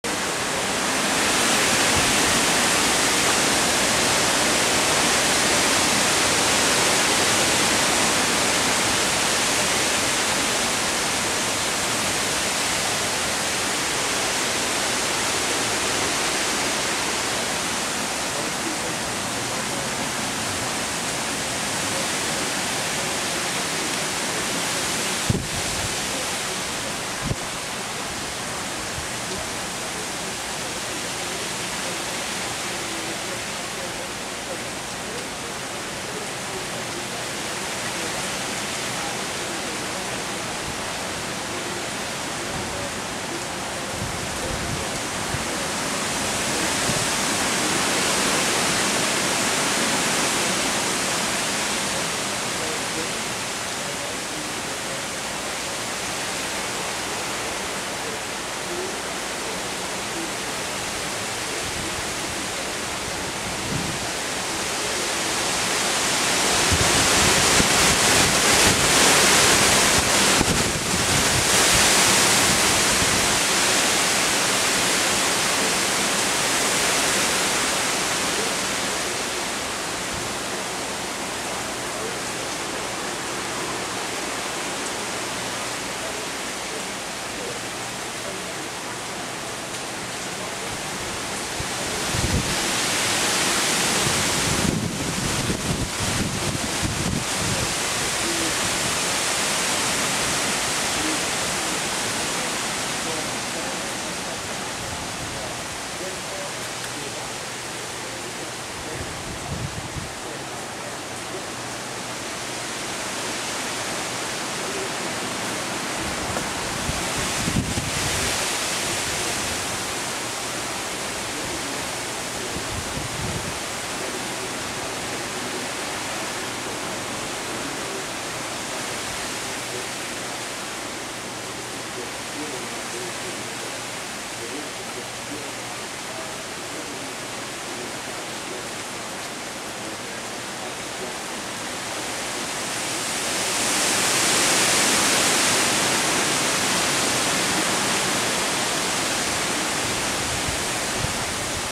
HURRICNE IRENE 2011: WINDS AND GUSTS PICKING UP
Hurricane Irene: Newport News VA around 10:00am Sat Aug  27, 2011
Winds and wind gusts picking up with sprinkles of rain.